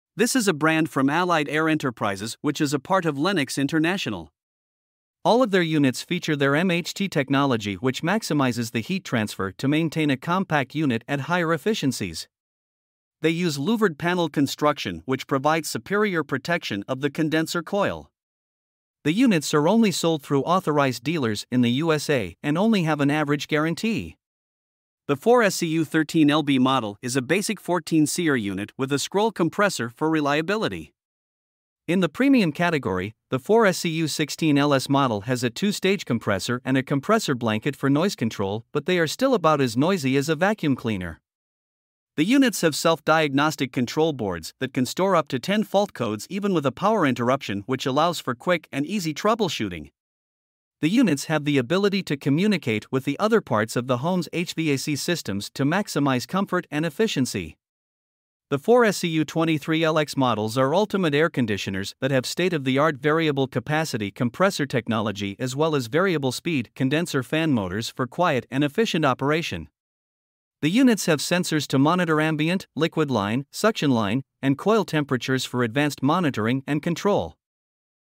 HvacRepairGuy 2023 Airease Brand Central AC Reviews
In this video, HvacRepairGuy reviews the 2023 Airease brand of central air conditioners. The reviews include the basic 4SCU13LB models, the premium 4SCU16LS units, and the ultimate 4SCU23LX models.

You can get the reviews of all of the brands